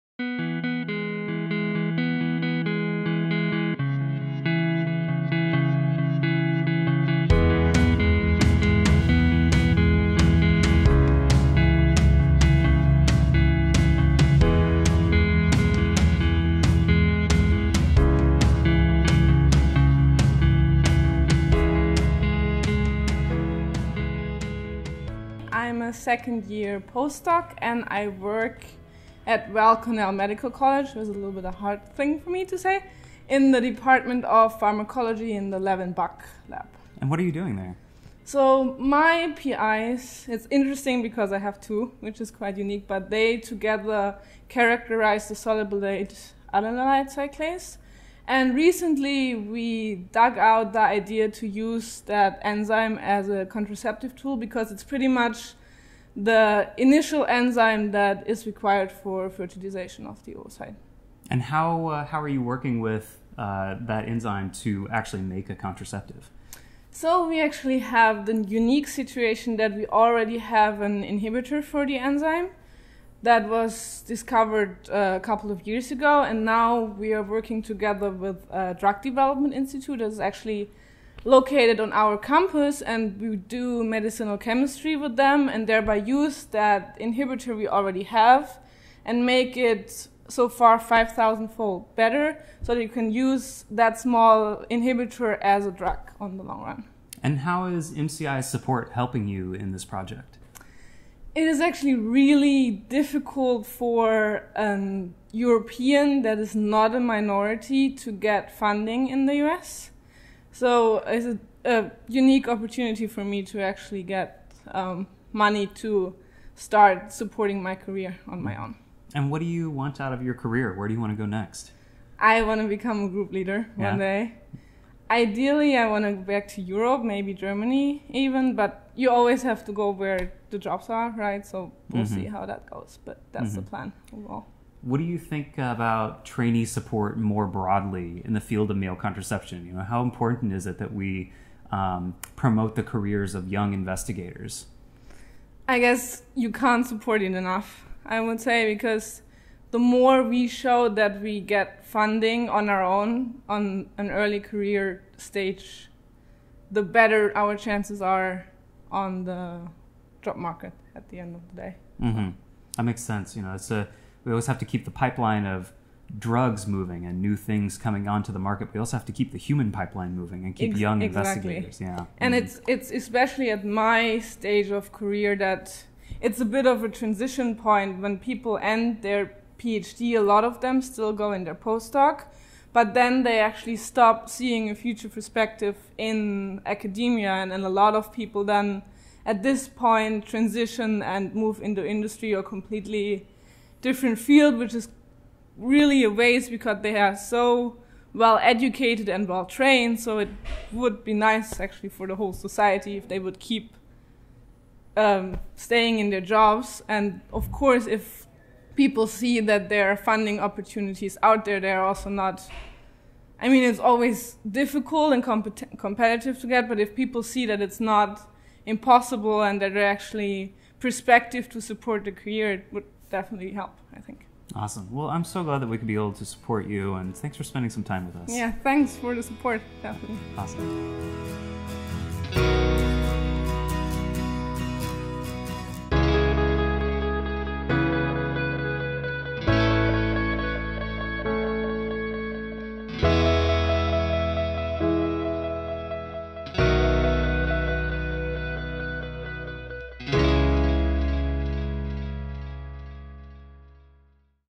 MCI Grantee & Fellow Conversations: Melanie Balbach, PhD
We met with MCI Fellow Melanie Balbach, a postdoctoral researcher at Weill Cornell in New York City, to discuss her work on soluble adenylyl cyclase (sAC), a protein essential for sperm motility and capacitation, her work across disciplines to develop an inhibitor of sAC that could be used as a male contraceptive, and how MCI’s Fellowship Program allows her to gain new skills in drug development.

This video shares highlights from that conversation.

For more information about Melanie and MCI's other fellows, please